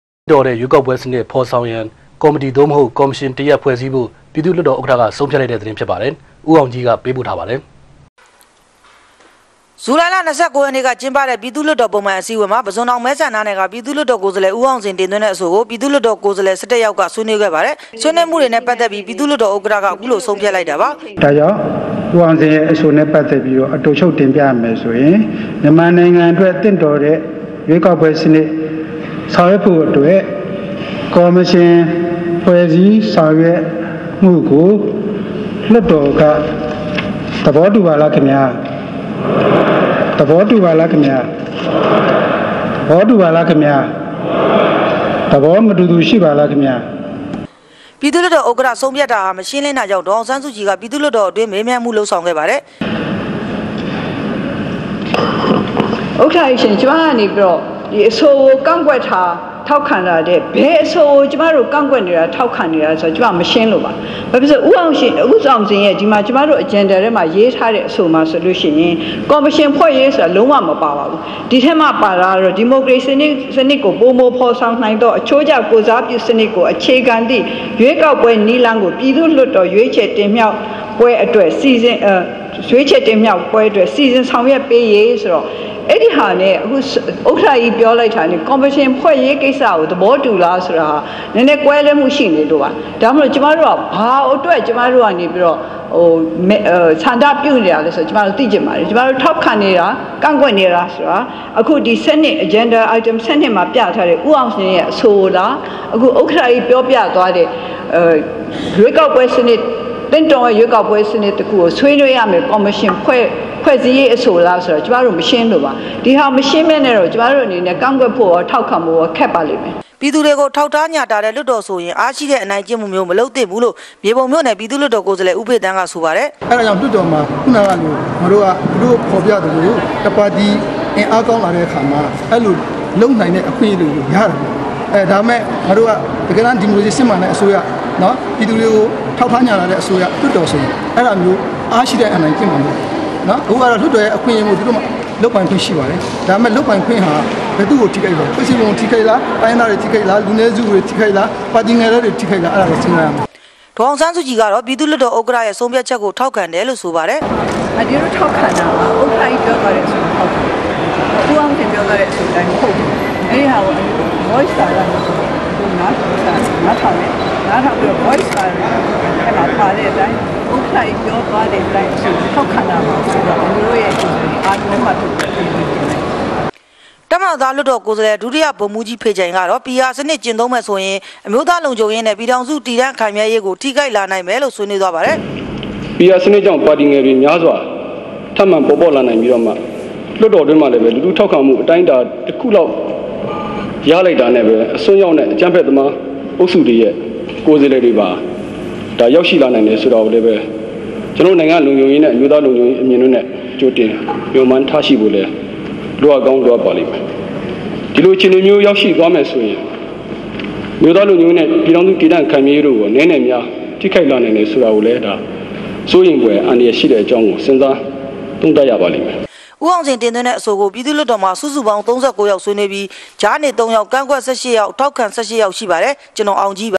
DVB - ေရြးေကာက္ပြဲစနစ္ ေဖာ္ေဆာင္ေရးအဖြဲ ့တခုုဖြဲ ့ဖိုု ့ ဆံုုးျဖတ္
ျမန္မာႏိုင္ငံအတြက္ သင့္ေတာ္တဲ့ေရြးေကာက္ပြဲစနစ္ ေဖာ္ဆာင္ရန္ ေကာ္မတီ သို႔မဟုတ္ ေကာ္မရွင္တရပ္ ဖြဲ႔စည္းဖို႔ကို ျပည္သူ႔လႊတ္ေတာ္ဥကၠဌက ဆံုးျဖတ္ လုိက္ပါတယ္။ ဦးေအာင္ၾကိးသတင္းေပးပို႔လိုက္ပါတယ္။ DVB TV - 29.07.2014